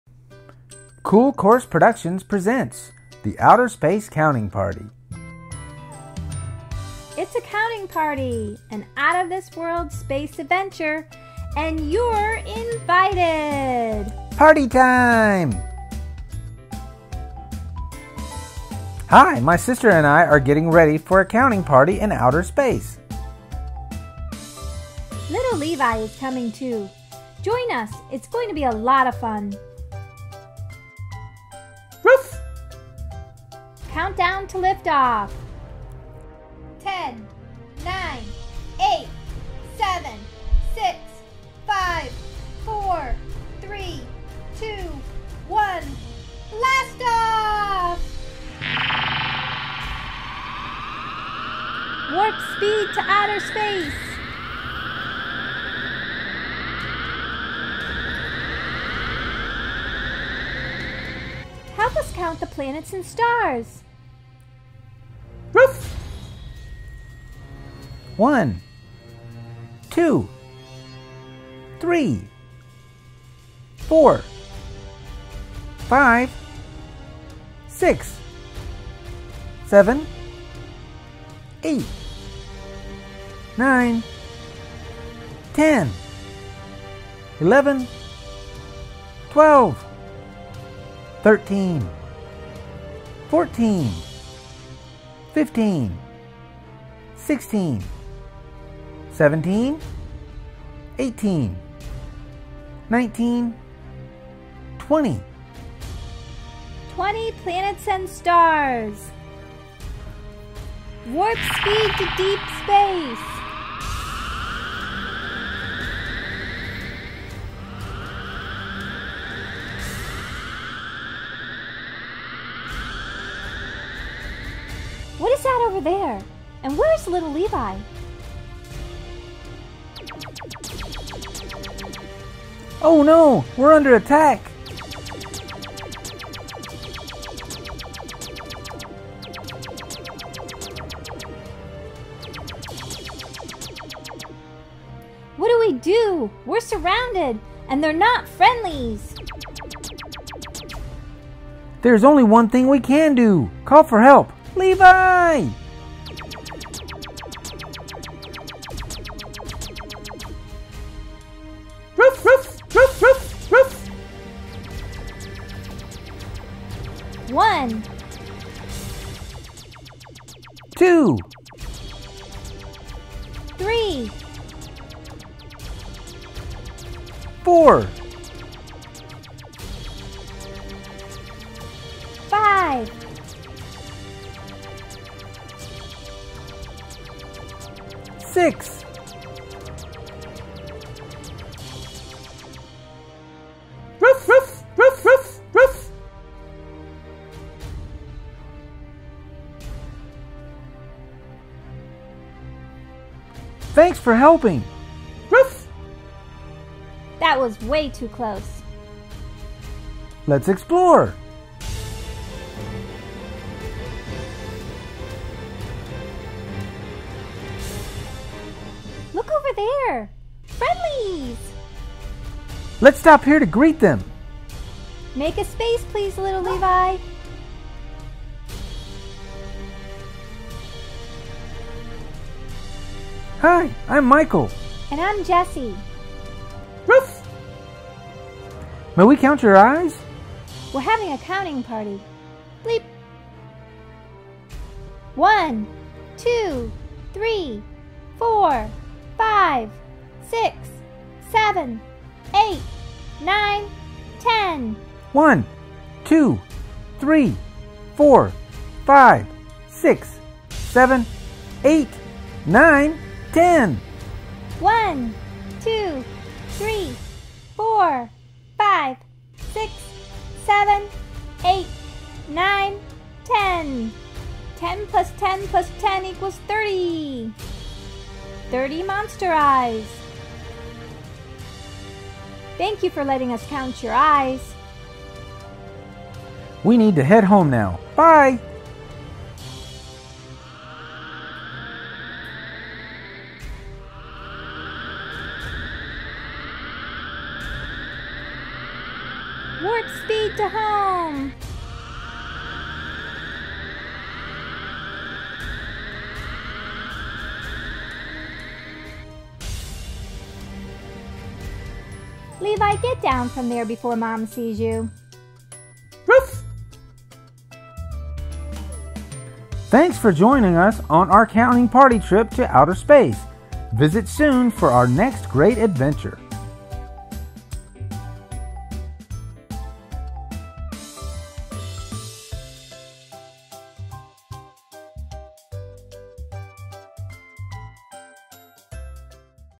THE OUTER SPACE COUNTING PARTY! | Counting to 10 | Learn to Count | Numbers Counting | Cool Course
In this cool course counting lesson, we travel through space, avoid alien capture, and count to 10 (sometimes 20) along the way! So kids, come along with us on the outer space counting party!!